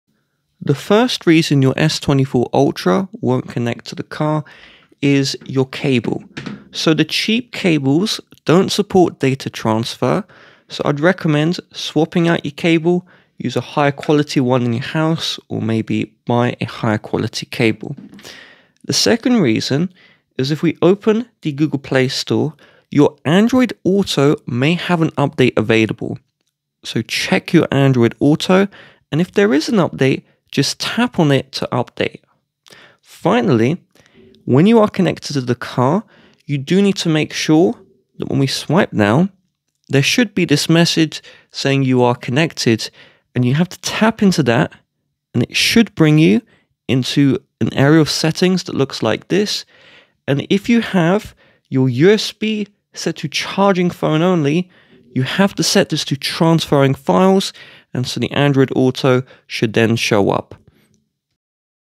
Samsung Galaxy S24 Ultra NOT Connecting to Car? SOLVED
Welcome to our troubleshooting guide for when your Samsung Galaxy S24 Ultra is not connecting to your car! In this video, we’ll explore solutions to address issues with car connectivity. We’ll cover steps such as checking Bluetooth settings, verifying car infotainment system compatibility, ensuring your phone and car are in pairing mode, and resetting Bluetooth connections. Follow along to resolve connectivity issues and get your Galaxy S24 Ultra working seamlessly with your car.